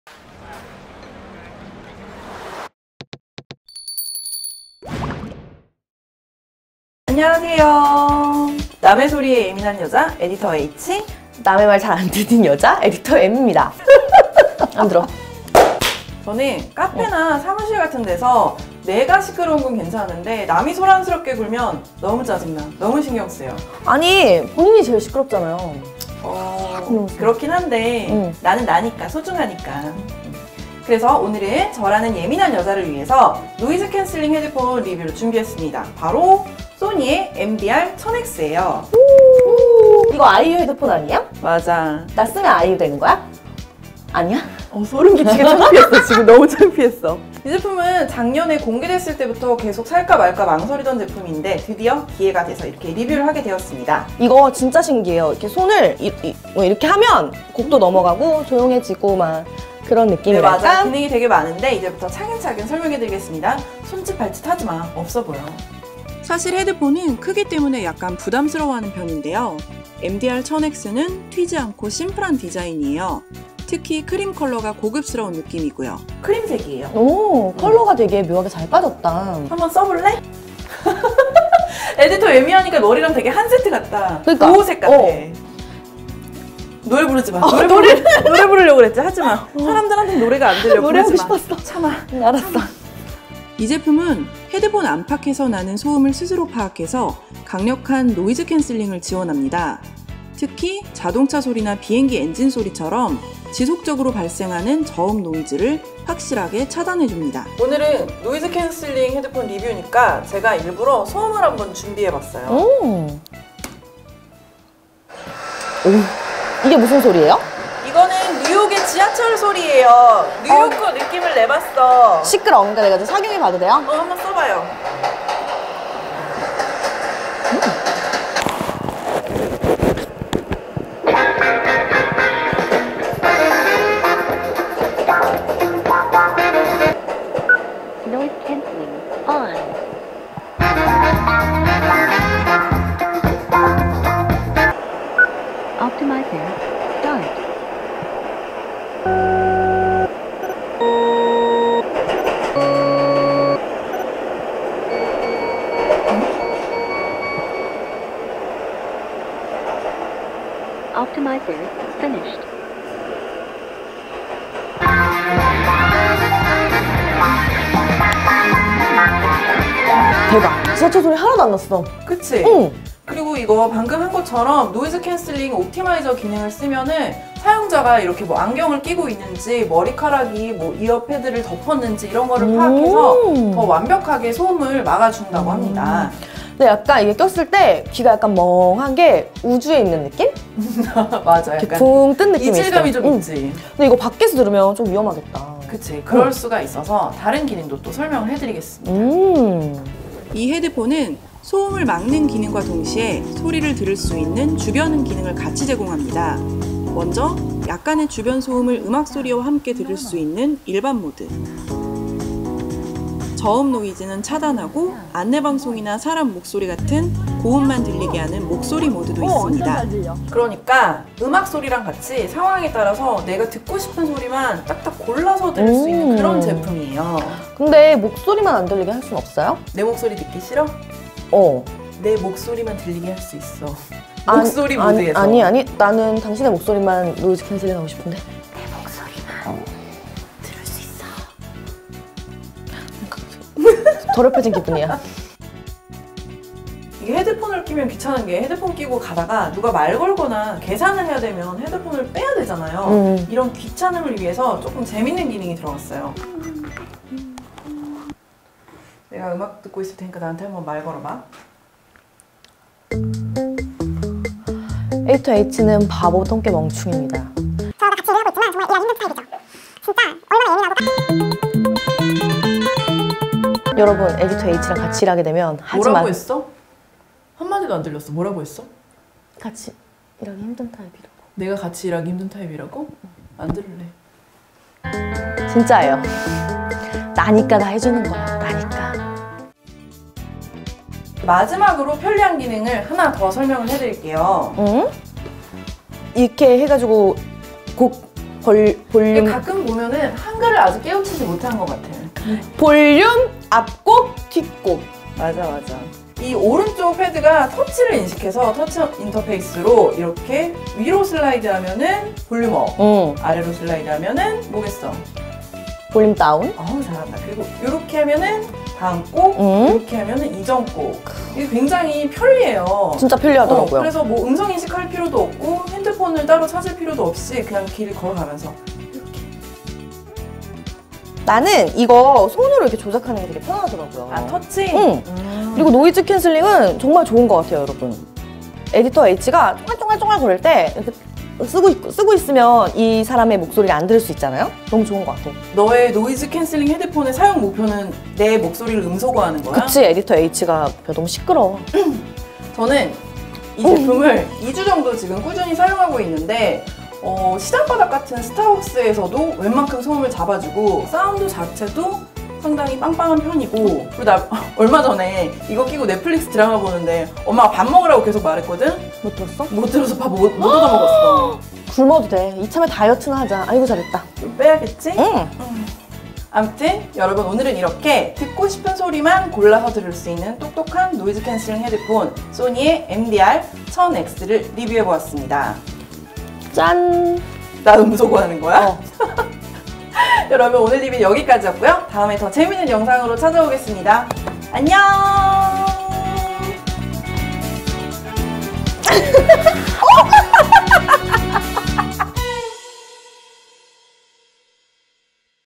듣고 싶은 소리만 듣는 아이유 헤드폰, MDR-1000X 리뷰
소니의 노이즈 캔슬링 헤드폰 MDR-1000X 롱텀 리뷰입니다.
노이즈캔슬링 기능이 어떻게 표현되는지 녹음하려고
엄청 애써서 촬영했으니 재밌게 봐주세요~
작년 신제품 발표회 현장에서 에디터M이 노이즈 캔슬링 기능을 켜고 큰 소리로
"나 아이유 같아?"라고 물어봐서 사람들이 다 쳐다본 건 비밀~^0^